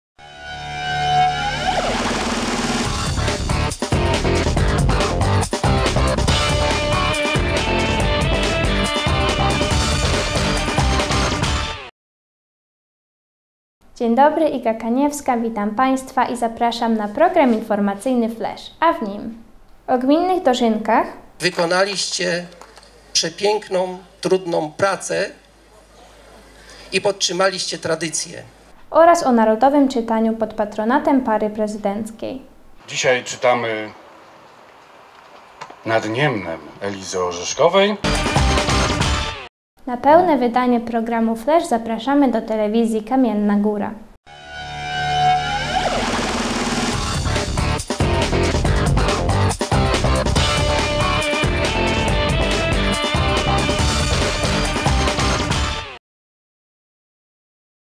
Program informacyjny Flesz 18.09.2023r.
- święto plonów w Gminie Marciszów
- Narodowe Czytanie w Kamiennej Górze